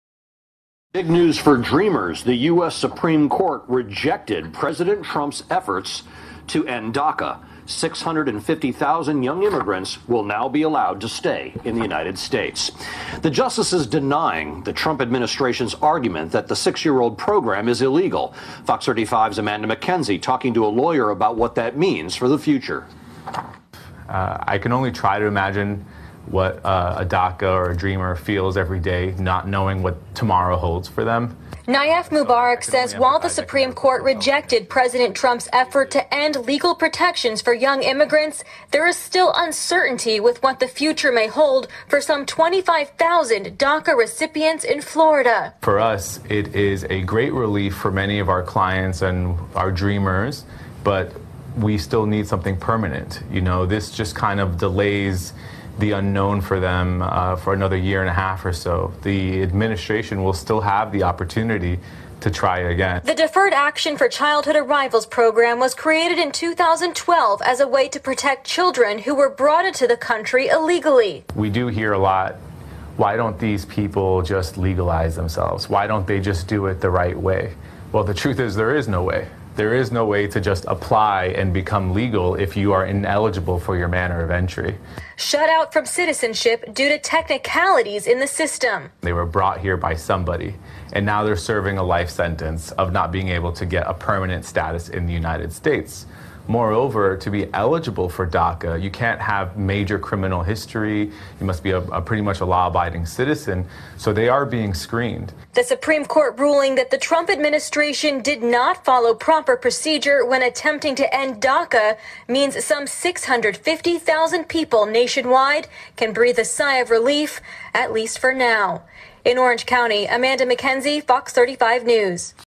Local immigration attorney reacts to DACA Supreme Court decision
- Orlando immigration attorney talks to Fox35 News about the Supreme Court's decision to reject President Trump’s effort to end legal protections for young immigrants. He also explains the need to find a permanent solution for the thousands of "dreamers."